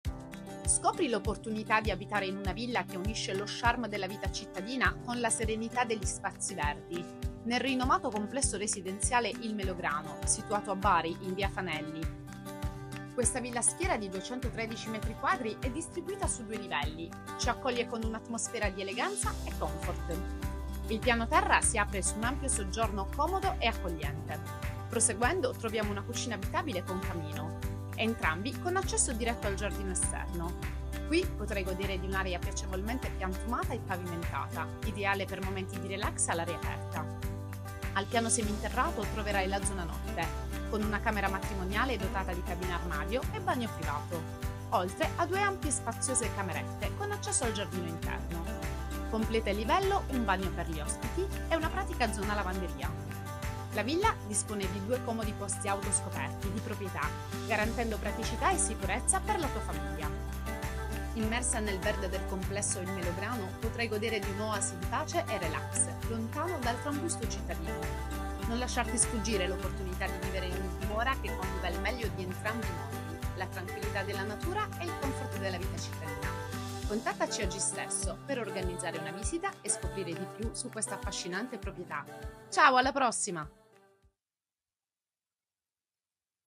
Bari, via Fanelli complesso il Melograno libera rifinita e comoda villa a schiera
su due livelli con piano terra composto da ingresso, soggiorno, cucina abitabile, bagno, vano lavanderia ed area scoperta parte piantumata e parte pavimentata con patio ed infine piano seminterrato composto da tre vani letto di cui il matrimoniale con cabina armadio e bagno privato, zona lavanderia e bagno ospiti. La villa dispone di due posti auto scoperti di proprietà. Soluzione ideale per chi predilige la tranquillità di un complesso residenziale alle porte della città che offre rilassanti spazi verdi e una silenziosa atmosfera.